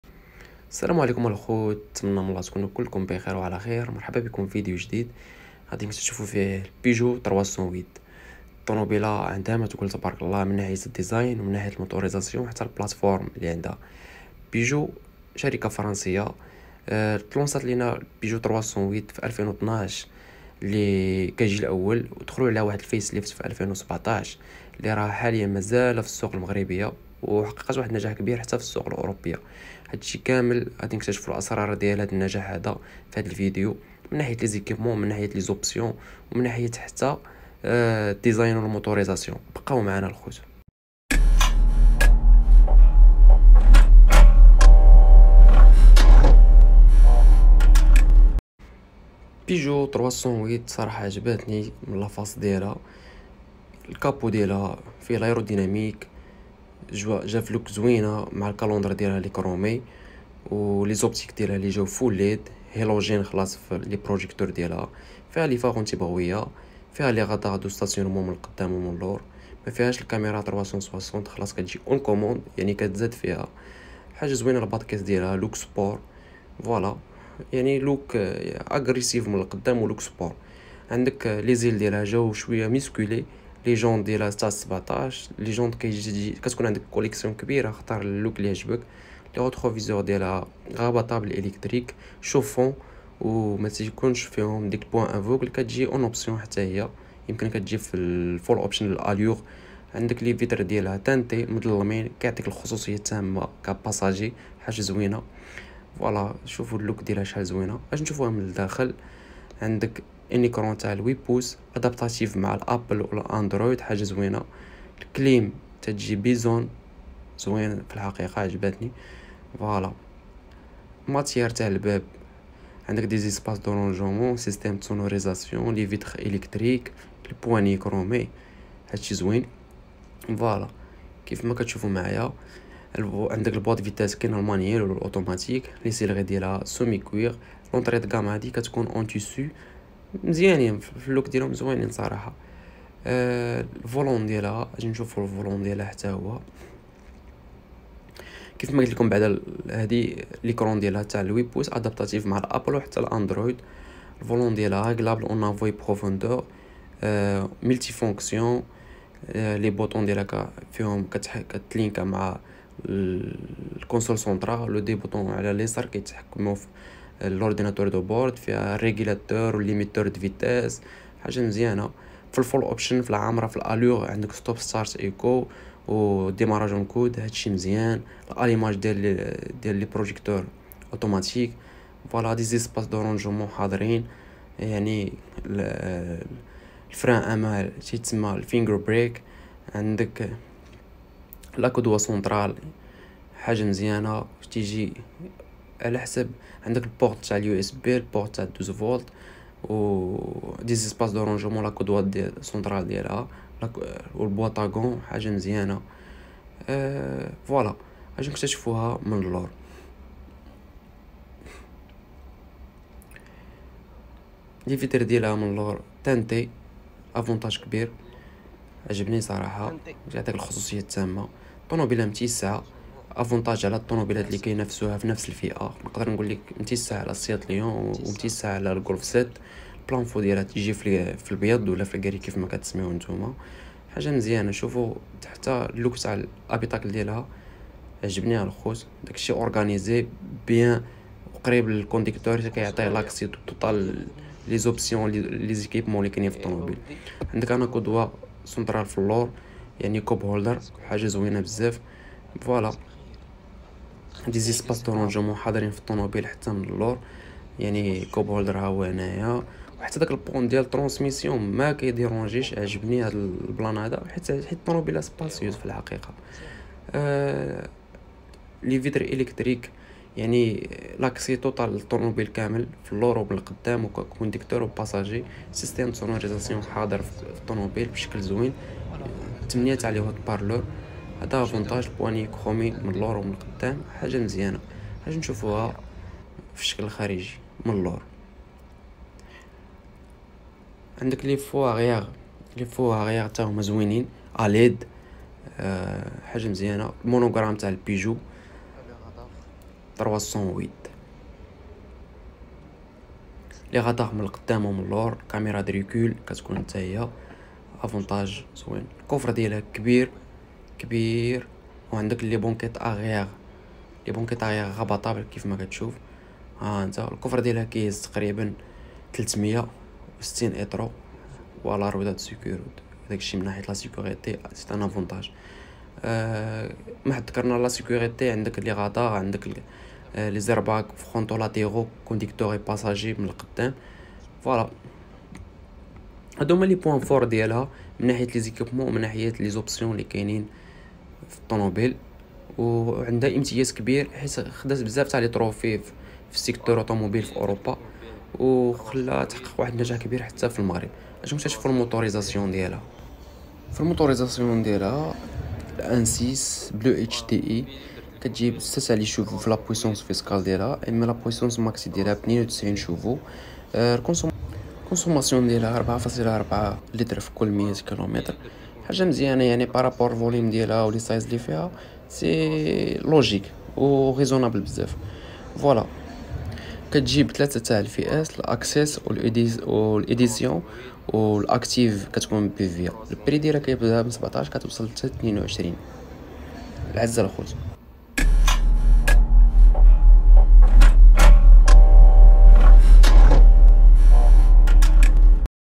Peugeot 308 2021 اجي نتعرفو على
اليوم قمت بالزيارة لوكالة الفرنسية البيجو Peugeot 308 فالحقيقة غني بمجموعة من الامتيازات ممكن قول منافسة لصناعة الالمانية و مكملة للفئة Peugeot 208 سيارة رياضية و شبابية مراجعة بالمغربية Maroc review